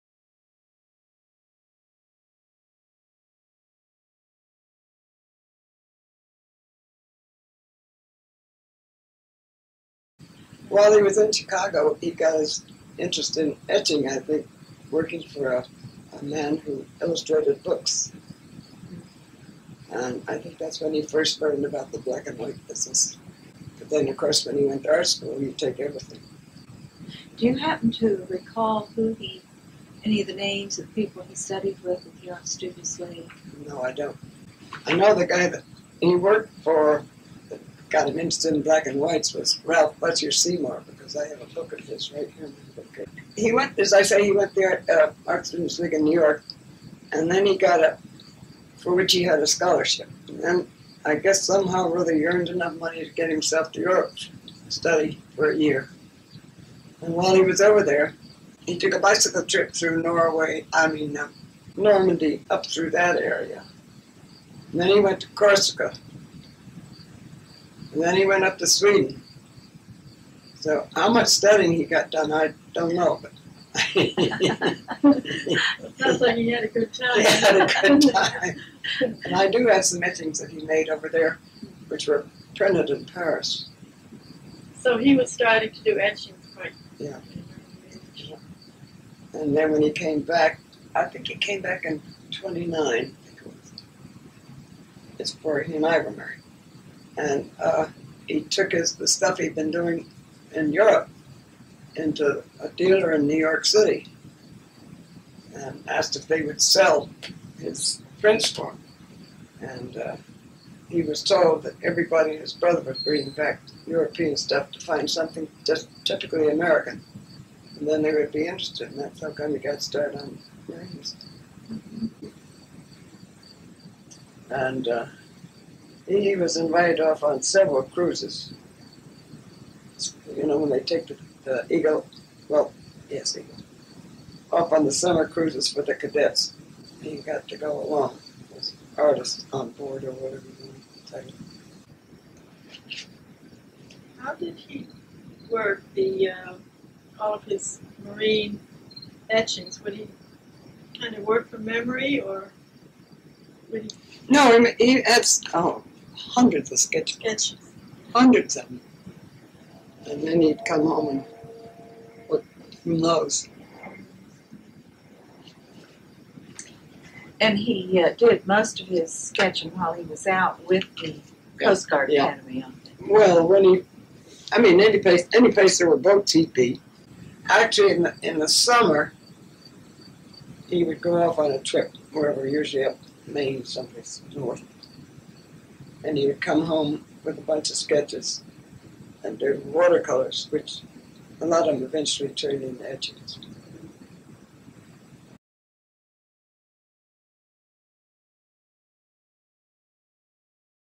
MMoA Archives: Yngve Soderberg - Etchings & Sketchbooks
Audio recording and interview with Nancy Soderberg (wife of Yngve Soderberg) by Lois H. Constantine and Priscilla Wright Pratt on September 2, 1992 at the artist's home in Mystic, CT.